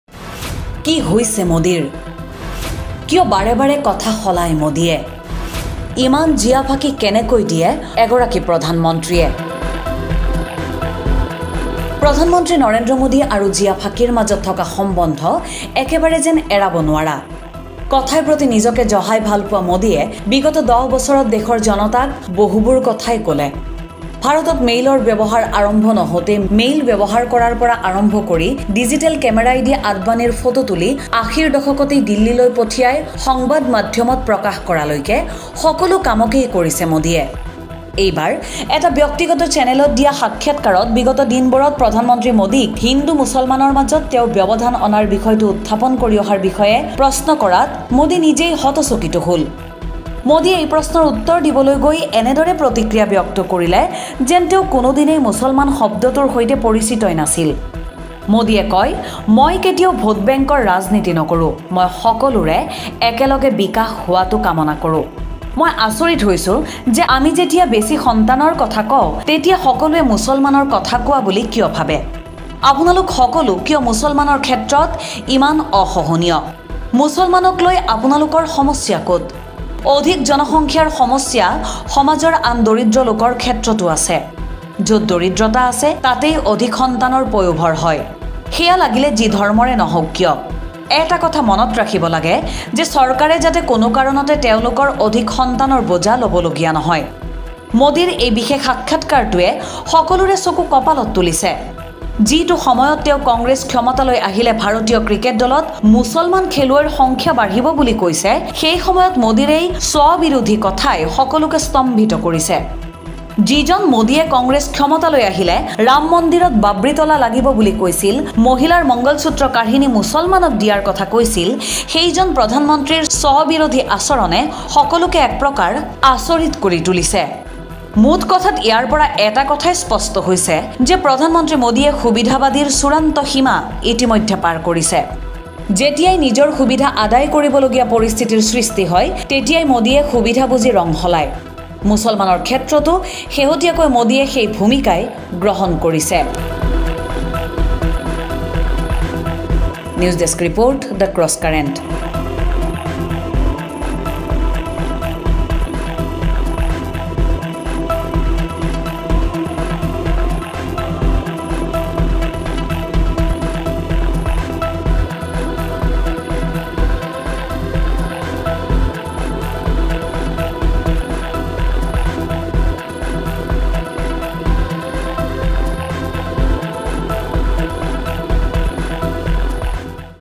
কি হৈছে মোডীৰ? কিয় বাৰে বাৰে কথা সলাই মোডীয়ে?
কি হৈছে মোডীৰ? কিয় বাৰে বাৰে কথা সলাই মোডীয়ে? ইমান জীয়া ফাঁকি কেনেকৈ দিয়ে প্ৰধানমন্ত্ৰীয়ে?